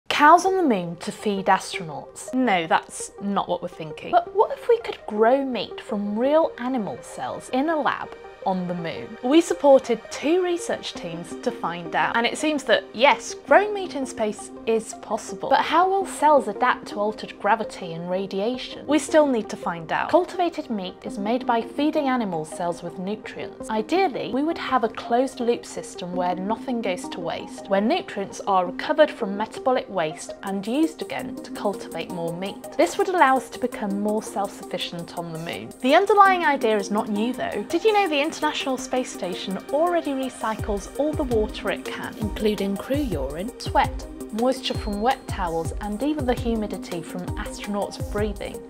Can we grow meat in space? 🥩
As we look ahead to long-term space missions, one of the big challenges is figuring out how to provide healthy and sustainable food for astronauts. To tackle this, ESA supported two research teams to investigate the possibility of cultivating meat in space.

🎥 ESA - European Space Agency